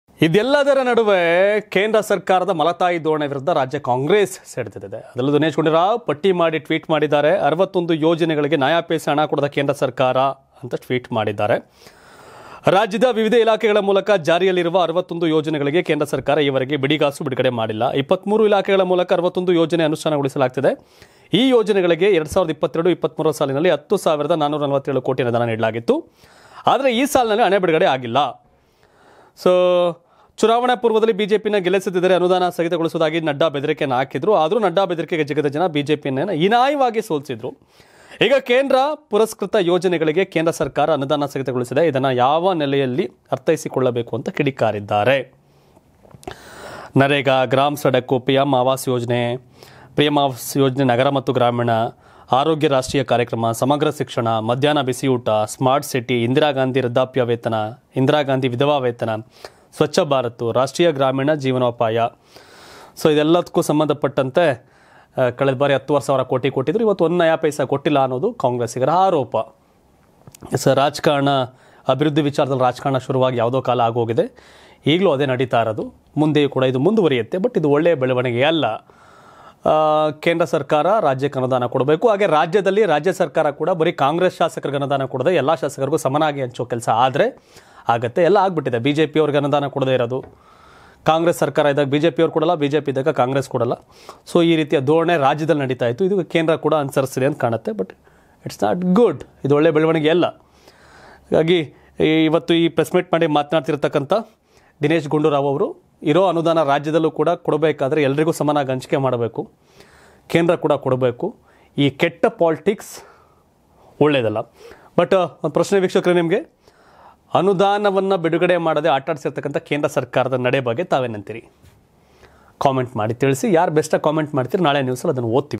61 ಯೋಜನೆಗೆ ಹಣ ಕೊಡದ ಕೇಂದ್ರ ಸರ್ಕಾರ JP Nadda ಹೇಳಿದ್ದು ಸತ್ಯವಾಯ್ತು ಎಂದ Dinesh Gundu Rao | Karnataka TV
61 ಯೋಜನೆಗೆ ಹಣ ಕೊಡದ ಕೇಂದ್ರ ಸರ್ಕಾರ JP Nadda ಹೇಳಿದ್ದು ಸತ್ಯವಾಯ್ತು ಎಂದ Dinesh Gundu Rao | Karnataka TV